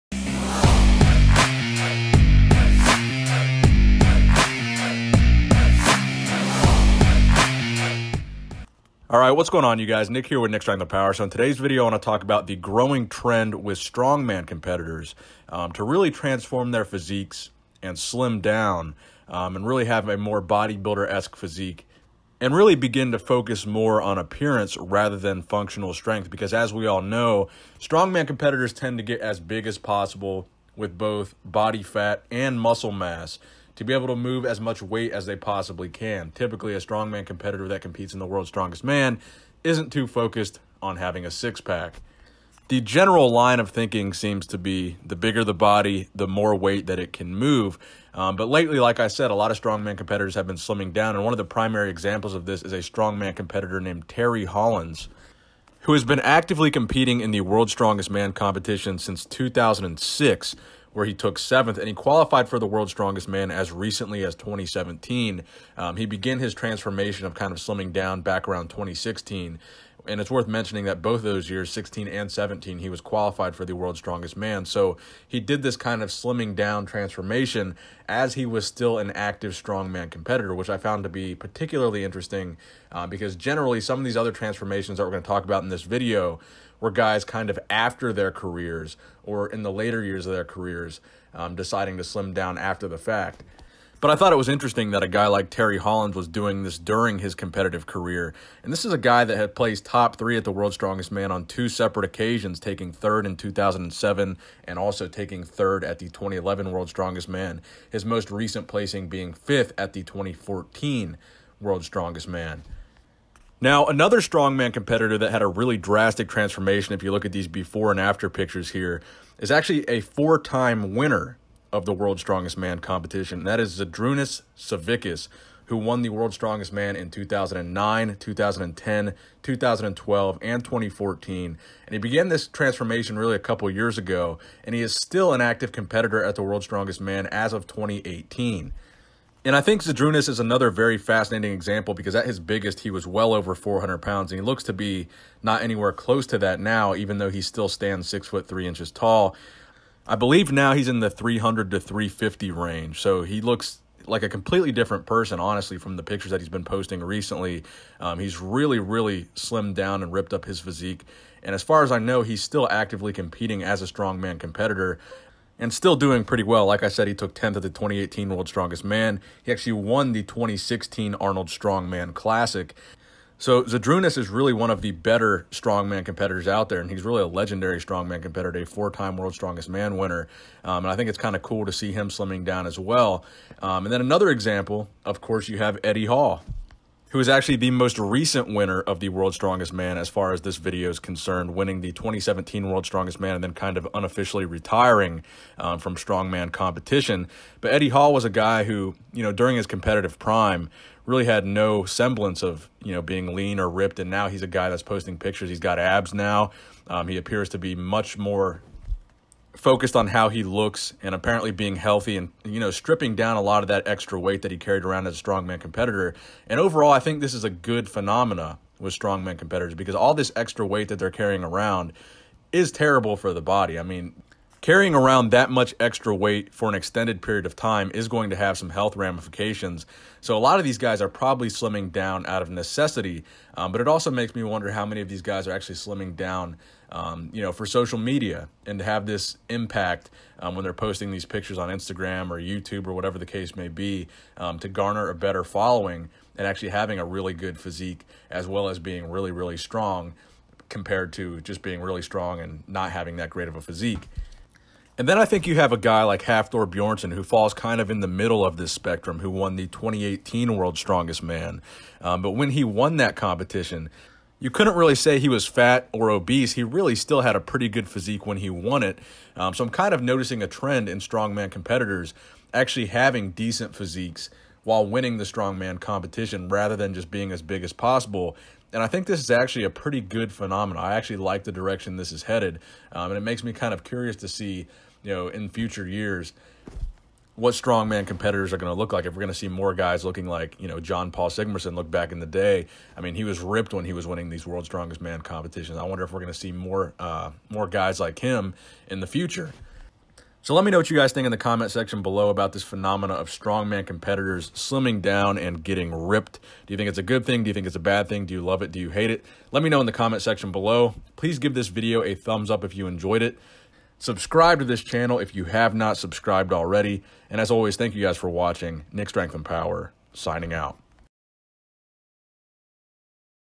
Strongman Competitors Getting Shredded
There's a growing trend among strongman competitors to transfrom their physique to a more bodybuilder-like physique.
*Interact with me on social media*